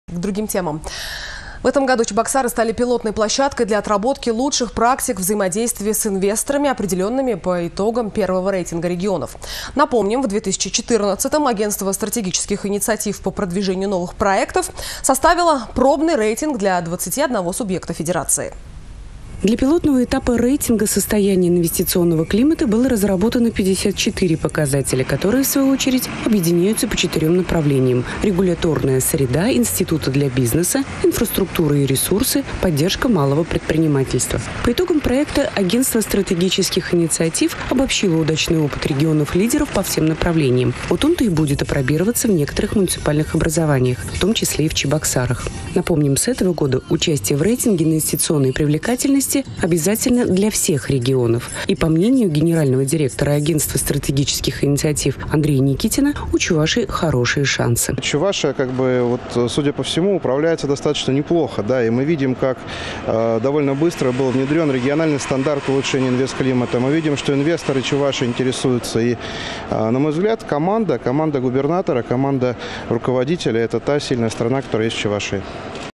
Чебоксары в пробном рейтинге Агентства стратегических инициатив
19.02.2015 Кстати, в этом году Чебоксары стали пилотной площадкой для отработки лучших практик взаимодействия с инвесторами, определенных по итогам первого рейтинга регионов. Напомним, в 2014 Агентство стратегических инициатив по продвижению новых проектов составило пробный рейтинг для 21-го субъекта федерации.